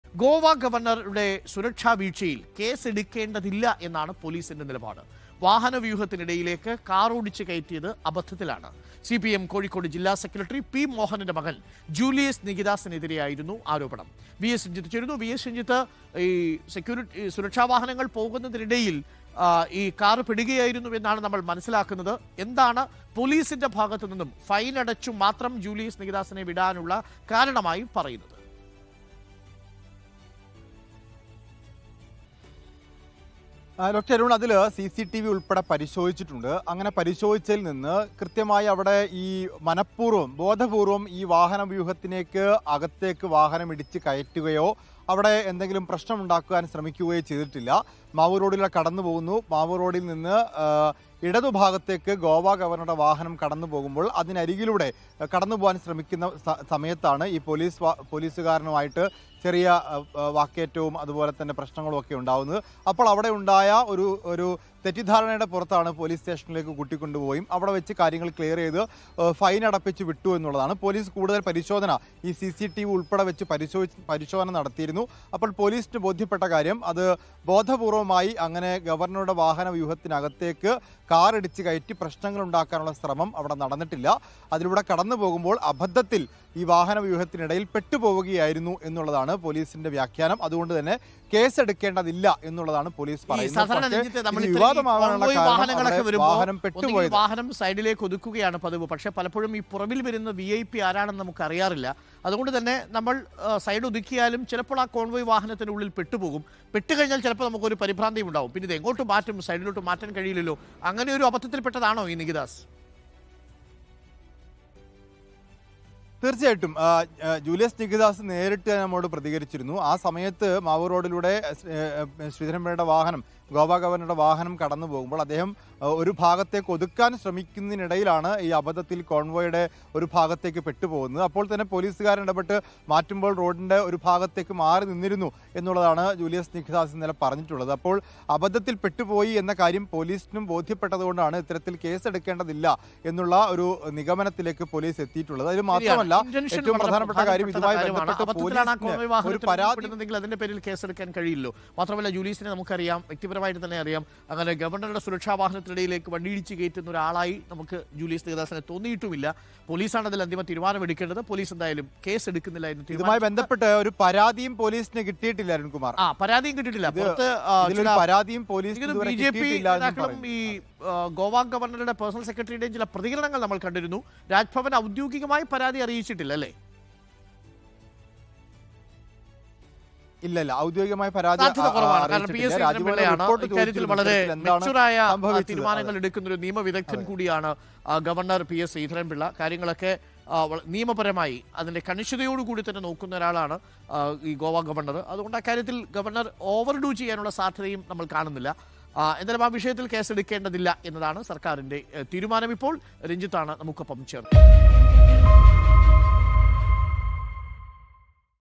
ഗോവ ഗവര്‍ണറുടെ സുരക്ഷാ വീഴ്ച്ചയില്‍ കേസെടുക്കേണ്ടതില്ലെന്ന് പൊലീസ് | Kerala Police
With Regards
Team RBC